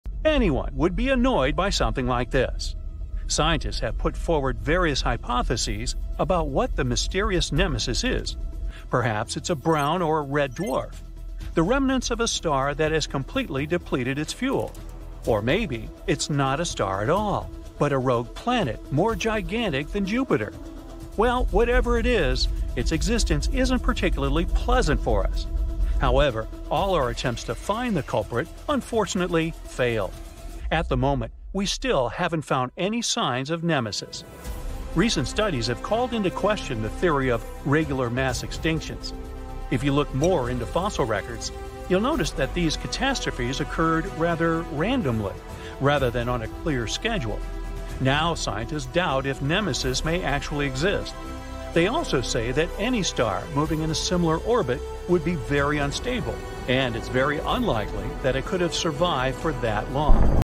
Nemesis: Is This Star Trying to Destroy the Sun? Space Secrets!
Join us as we delve into the intriguing mystery of Nemesis, the hypothetical star threatening our Sun! We explore various theories about Nemesis's motivation and nature, analyze the evidence challenging the mass extinction theory, and examine why its existence is now doubted by scientists. Discover captivating space facts and the latest scientific findings in this mind-blowing exploration.